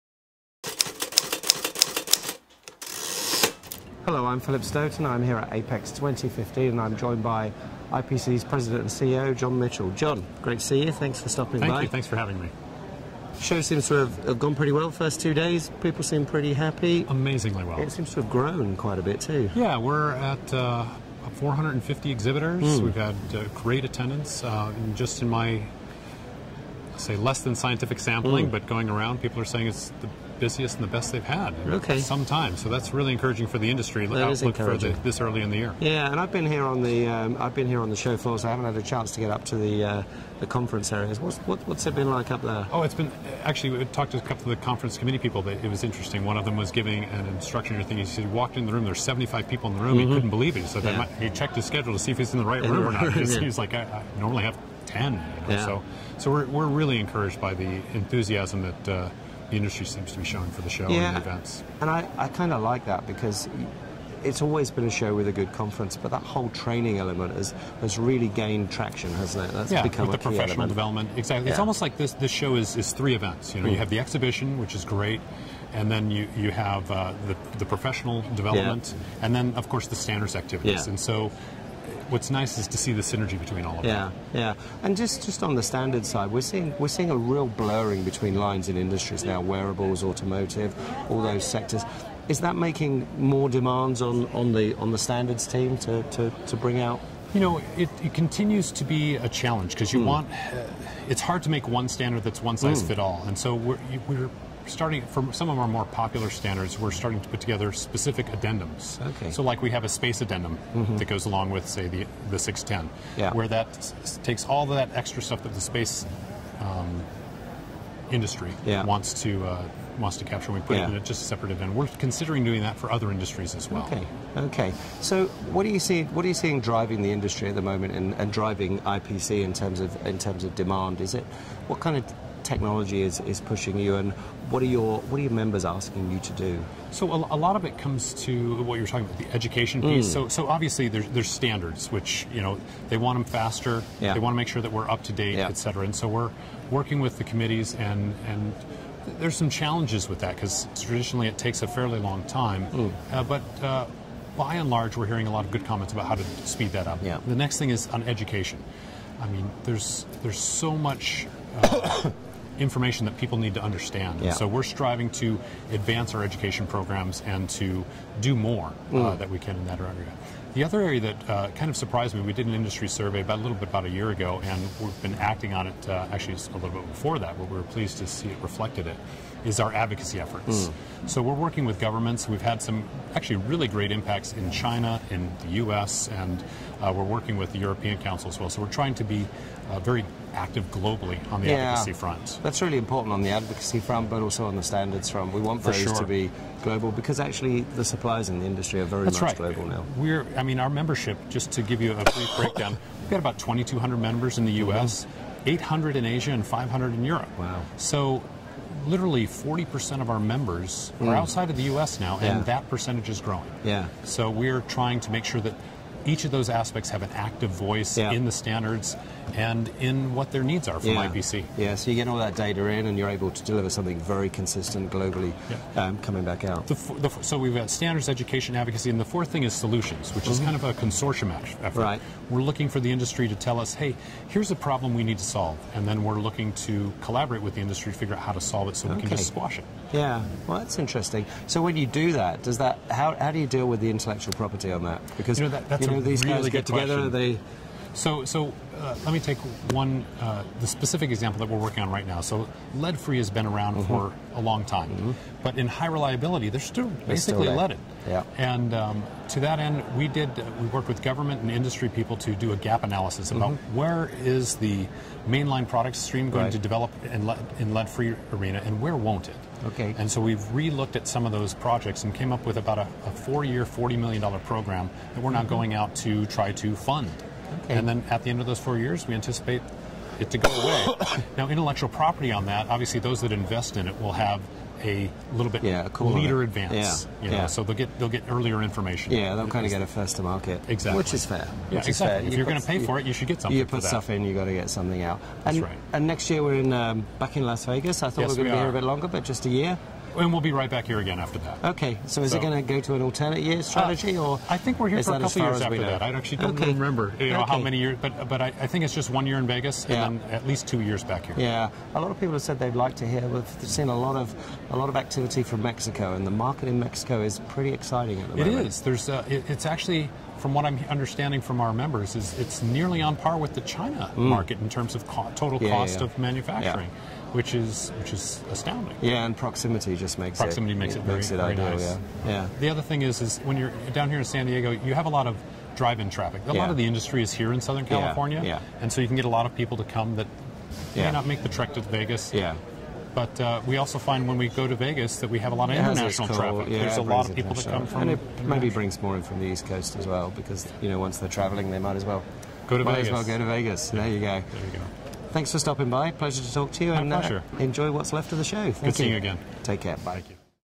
IPC President & CEO, John W. Mitchell at APEX 2015
Philip Stoten (@philipstoten) and IPC's President & CEO John W. Mitchell review IPC APEX exhibition and discuss the latest feedback from the exhibition and conference program. Filmed live on location in San Diego at IPC APEX 2015 by @thescoop.